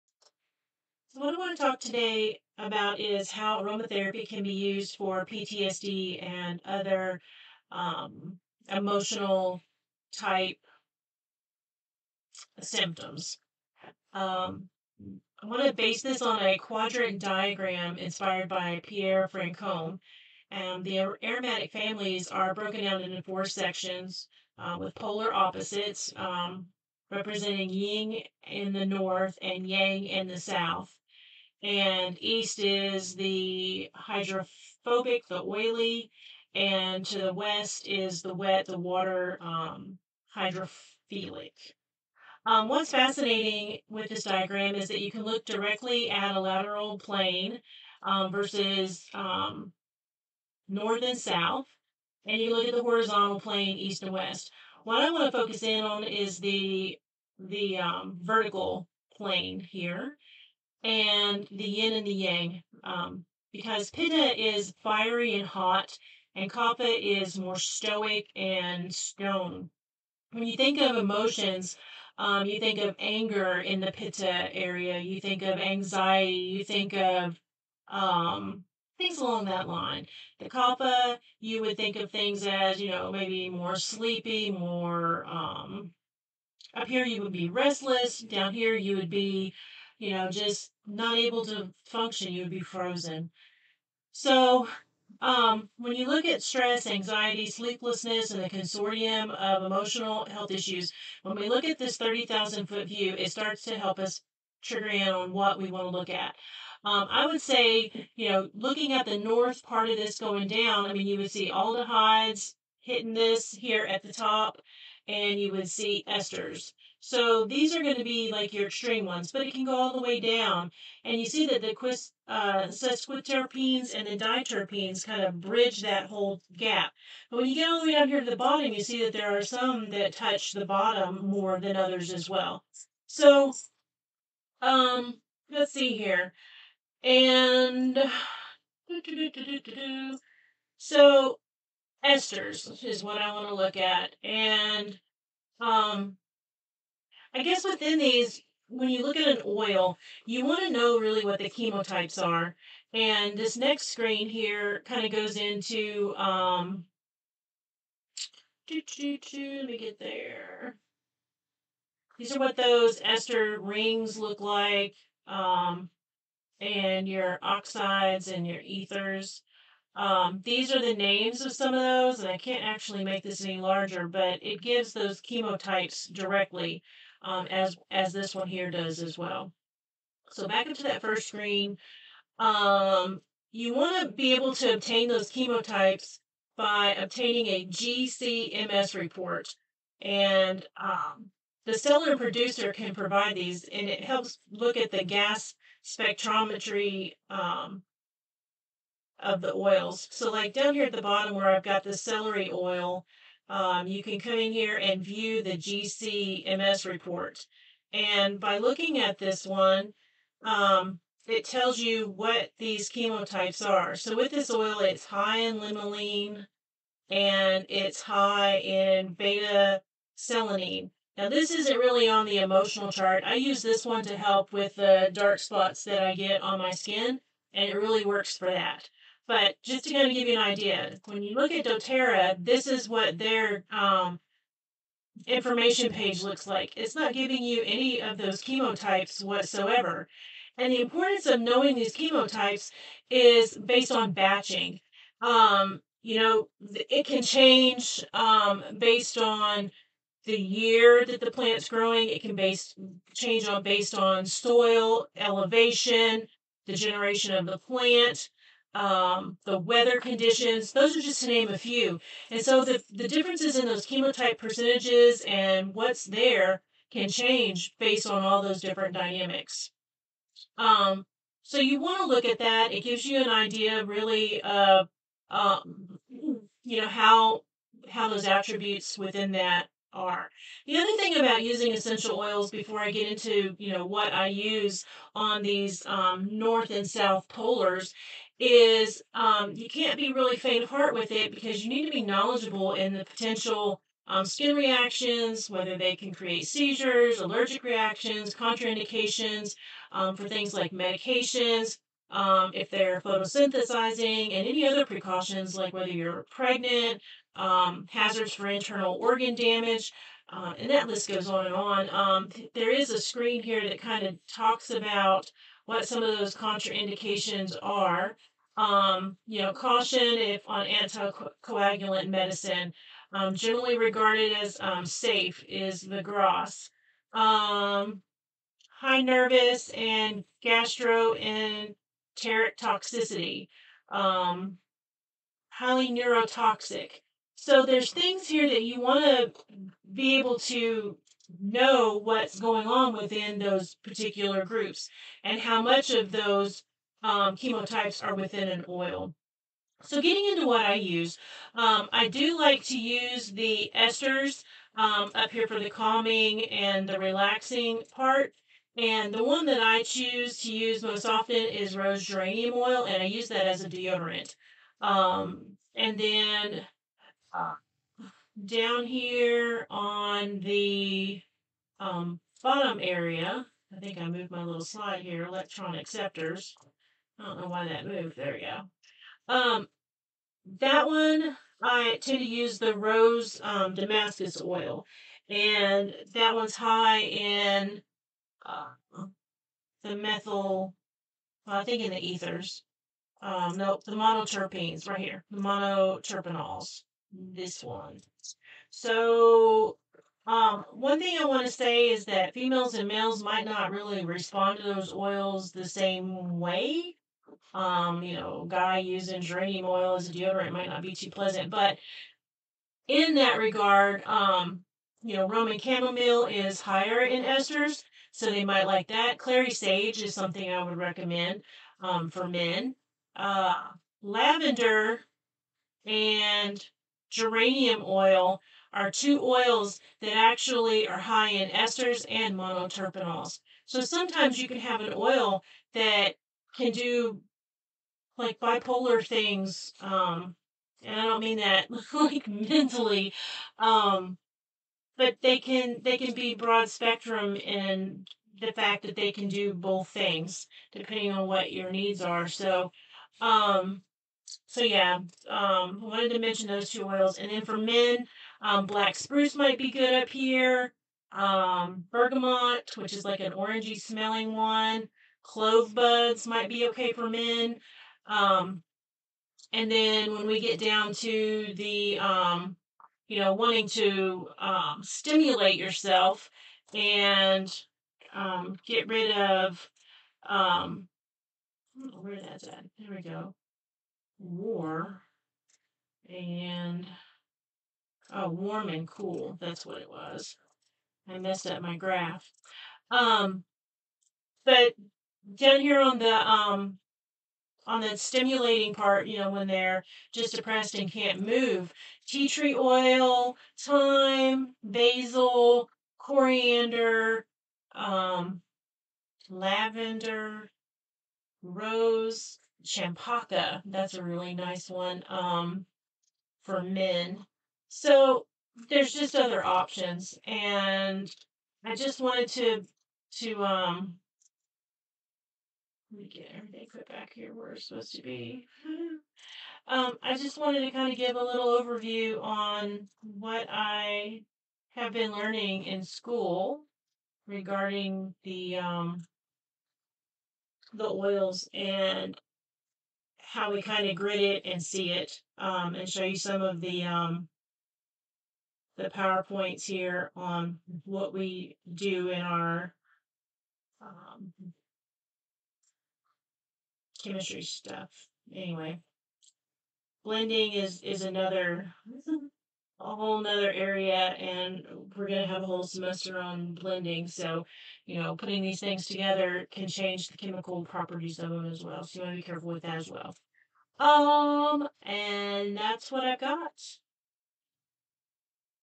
Aromatherapy and Emotional Support
How Chemotypes of oils and obtaining a GCMS report per batch is critical and how the emotional plane is impacted. Recommendations:
1.)  Oils high in Esters for calming agressive/anxiety type emotions:
           Chamaemelum nobile (70-80%) – Roman Chamomile
           Salvia sclarea (65-80%) – Clary Sage
           Helichrysum italicum (45-70%) - Helichyrsum
           Citrus aurantium, leaf (60%) – Neroli
       *  Lavandula angustifolia (50%) - Lavender
           Citrus bergamia (30-40%) - Bergamot
           Picea mariana (30-37%) – Black Spruce
       *  Pelargonium x asperum (20-33%) – Rose Geranium Oil
           Eugenia caryophyllus (22%) – Clove Bud
           Citrus aurantium flower (7-21%) - Neroli

2.)   Oils high in Monoterpenols for stimulating a stoic solid consititution:
           Aniba rosaeodora (95%) - Rosewood
           Cinnamomum camphora linaloliferum (95%) - Ravinstra
           Cymbopogon martinii (80-85%) - Palmarosa
           Coriandrum sativum (60-80%) - Coriander
           Thymus vulgaris linaloliferum (75%) - Thyme
           Rosa damascena, distilled (65-75%) - Rose
           Michelia champaca alba,absolute (67%) - Champaka
      *   Pelargonium x asperum (50-65%) – Geranium Oil
           Thymus vulgaris thujanoliferum (54-60%) - Thyme
           Ocimum basilicum linaloliferum (40-60%) - Basil
           Thymus satureioides (28-50%)
           Origanum majorana (38-45%) - Marjoram
           Melaleuca alternifolia (30-45%) – tea tree
           Mentha x piperita (40-45%) - peppermint
     *    Lavandula angustifolia (30-40%) - Lavender
           Agonis fragrans (33%) – Fragonia

Men and women may not prefer the same types of oils. I personally prefer Rose Geranium, and it's GREAT because it is one of 2 that can both calm and stimulate (the other is lavender).